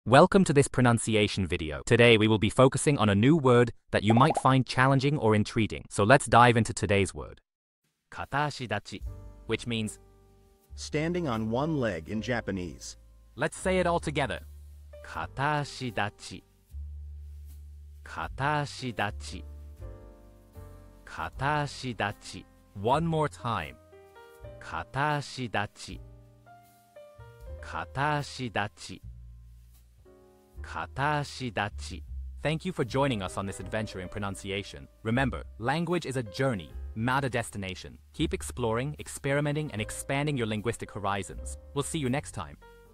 How to pronounce kataashitachi | 片足立ち (Standing on one leg in Japanese)
Master the Pronunciation of 'kataashitachi | 片足立ち' - which means : Standing on one leg in Japanese 📝 with @PronunciationJapanese 🎓🗣️ - [Your Guide to Japanese]

Learn to pronounce 'kataashitachi' perfectly in Japanese with our easy and engaging tutorial by @PronunciationJapanese! 📖📽️ In this comprehensive video, we provide you with native speaker examples 🗣️👂 and phonetic breakdowns 🎵 to ensure you get the pronunciation just right.